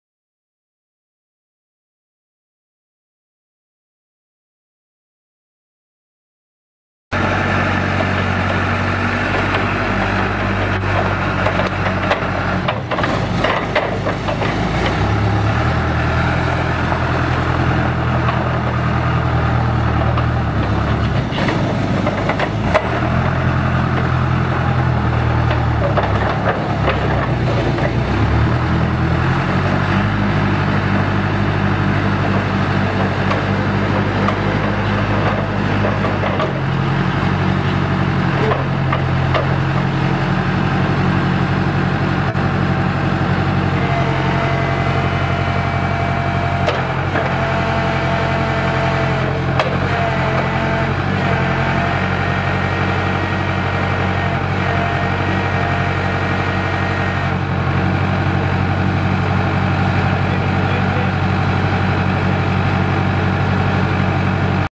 John Deere 482C forklift for sale | sold at auction May 14, 2015
John Deere 482C forklift
4F-4R gears
5,000 lbs maximum lift capacity
48"L x 4"W forks
Lift, tilt, and side shift
19.5L-24 front tires
11L-16 rear tires
11,532 lbs. base operating weight
Serial 60761

It was located in Gardner, KS, and we sold it to the highest bidder for $9,790.